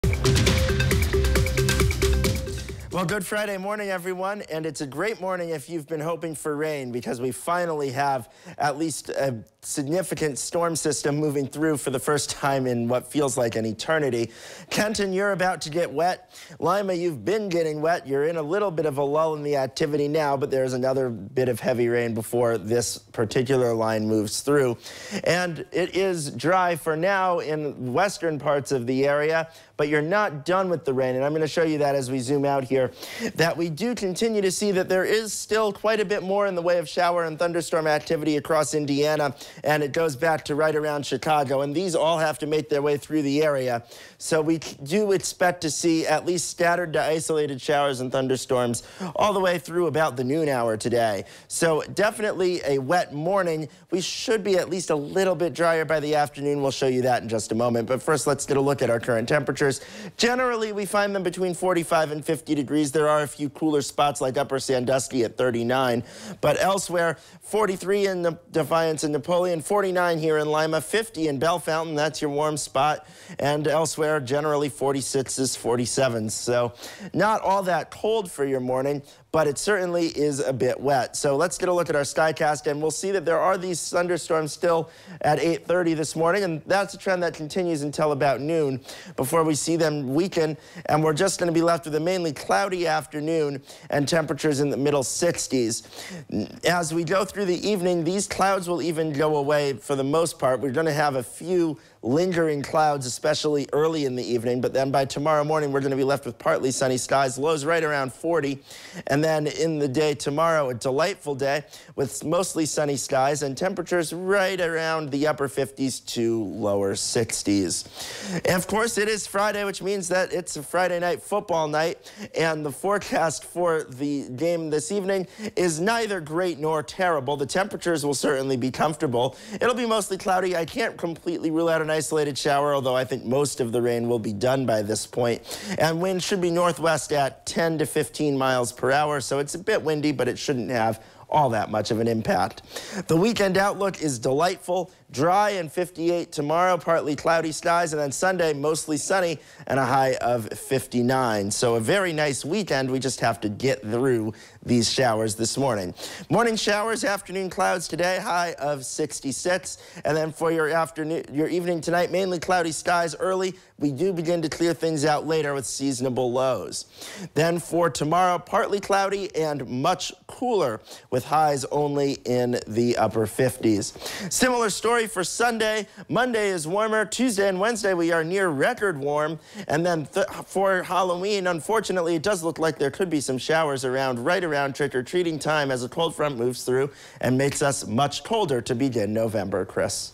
Oct 25 AM Wx
Thunderstorms are rolling through the area today, but we should dry out for the weekend. Near record highs are possible by next week!